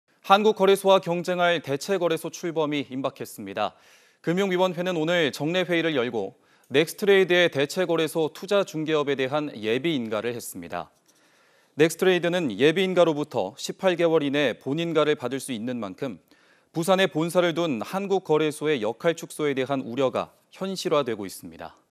대체거래소 출범 임박, '넥스트레이드' 예비인가 통과
한국거래소와 경쟁할 대체거래소 출범이 임박했습니다.

금융위원회는 오늘(19) 정례회의를 열고 '넥스트레이드'의 대체거래소 투자중개업에 대한 예비인가를 했습니다.

넥스트레이드는 에비인가로부터 18개월 이내 본인가를 받을 수 있는 만큼 부산에 본사를 둔 한국거래소의 역할 축소에 대한 우려가 현실화되고 있습니다.